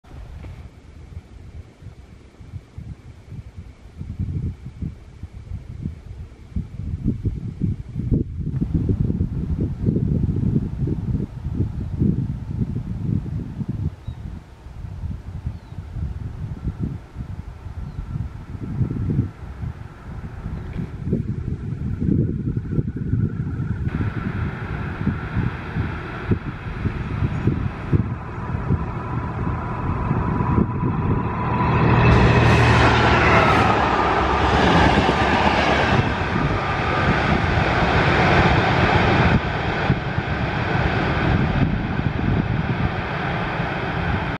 Diagnostická jednotka DJ NDT - 2.11.2023
Diagnostická jednotka DJ NDT (Správa železnice) projíždí po skončení výluky zastávkou Tatenice ve směru Česká Třebová. Natočeno ve čtvrtek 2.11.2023 v 12.09 hodin.